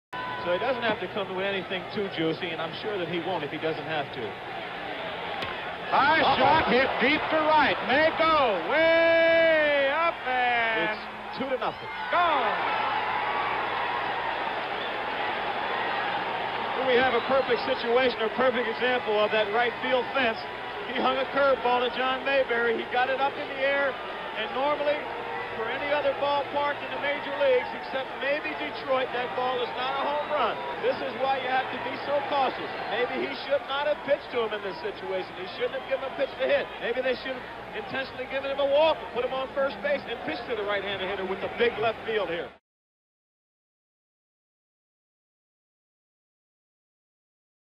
1976 ALCS Gm5: Mayberry's homer gives Royals 2-0 lead
10/14/76: John Mayberry's two-run home run in the top of the 1st gives the Royals an early 2-0 lead over the Yankees in Game 5